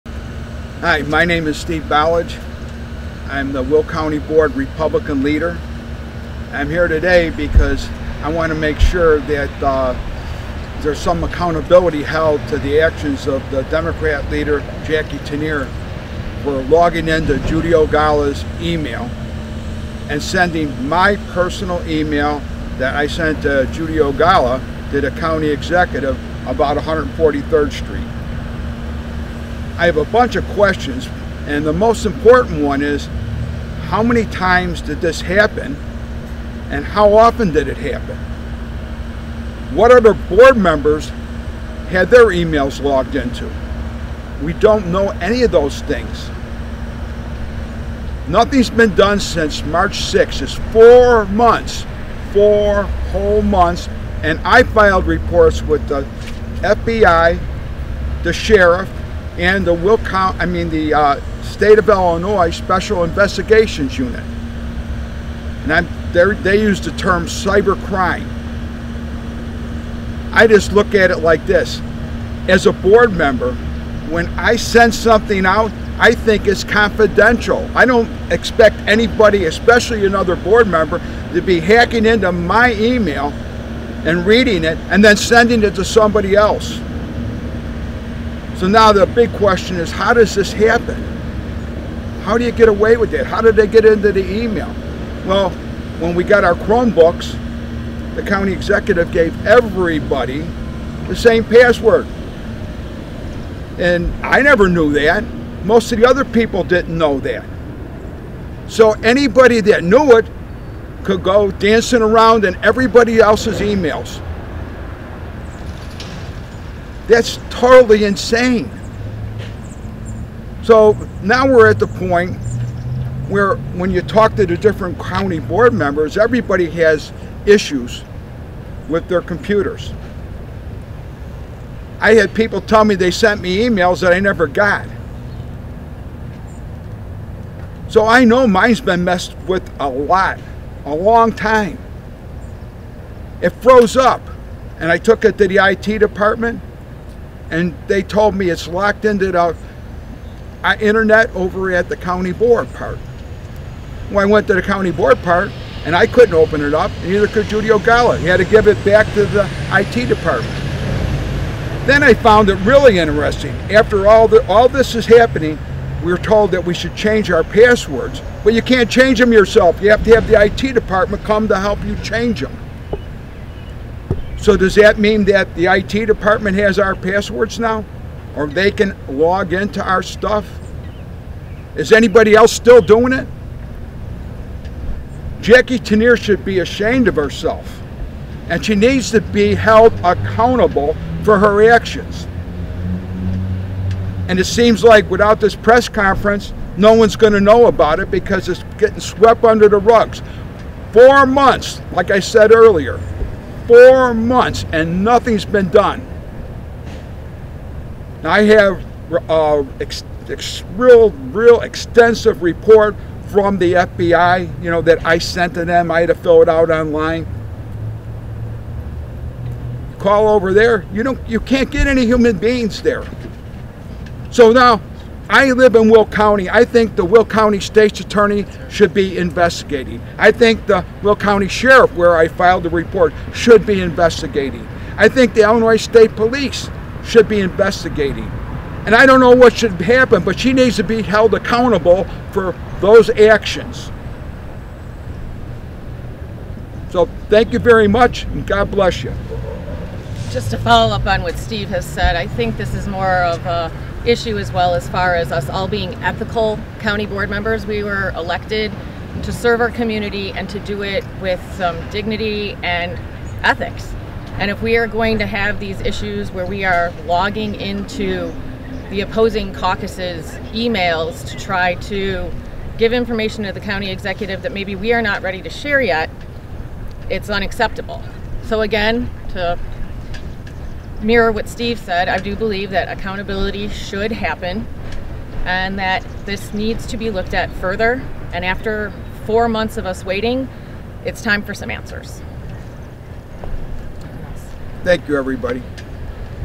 Balich holds news conference over alleged cyber crime by political opponent
Steve Balich holds news conference, at the Will County Office building, over ‘alleged cyber crime’ committed by Will County’s Democrat Leader.